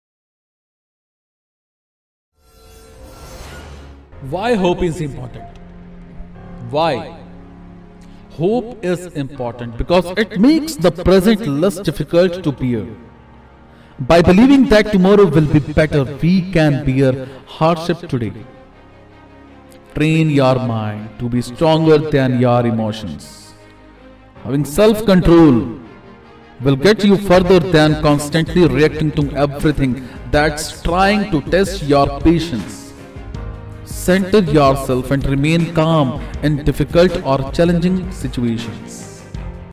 Why hope is important - Motivational Video by Rajan Chaudhary - Morning Motivation
Hope is important because it makes the present less difficult to bear. By believing that tomorrow will be better we can bear hardship today.

Train your mind to be stronger than your emotions. Having self-control will get you further than constantly reacting to everything that’s trying to test your patience. Center yourself and remain calm in difficult or challenging situations.

Some related videos links :-

Shall we begin to give and remove and spread every atom of goodness inside us | Rajan Chaudhary